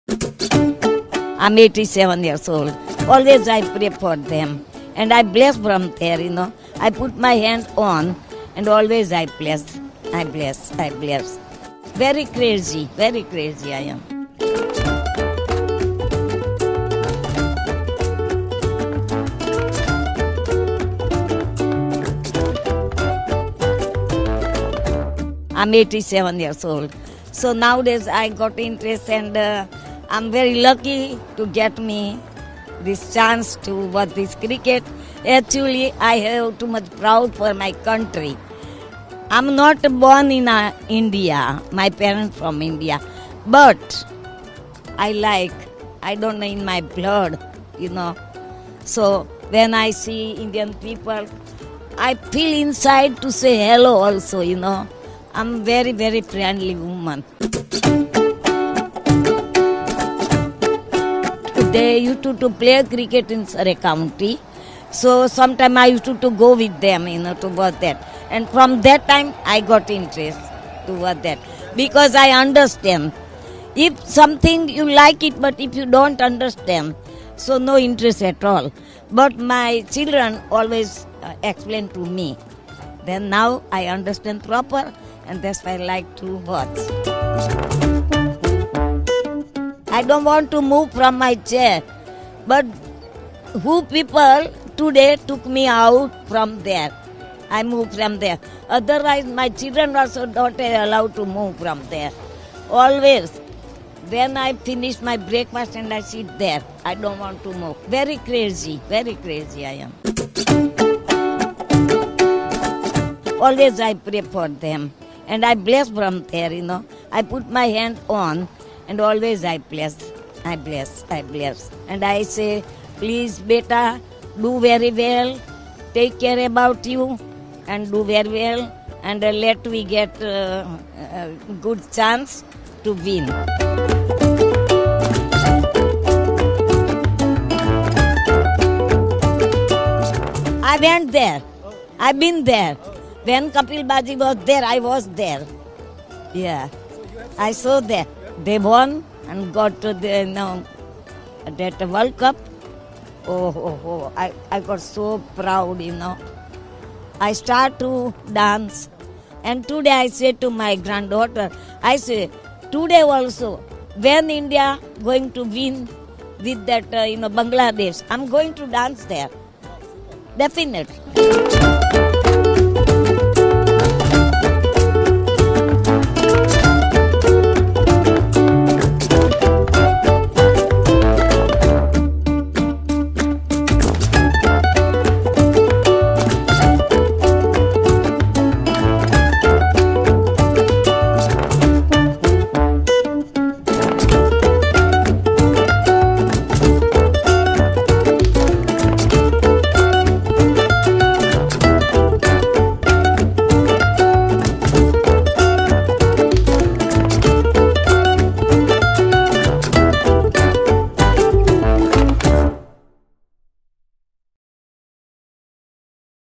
Ind vs Ban: Team India’s 87-Year-Old Fan Steals the Show at Edgbaston
ICC World Cup 2019: On Tuesday, while Rishabh was busy smashing boundaries against Bangladesh at Edgbaston, Team India found a new fan in the form of an elderly lady. The passionate fan, identified as 87-year-old Charulata Patel, was seen cheering from the stand, blowing a trumpet with full zeal.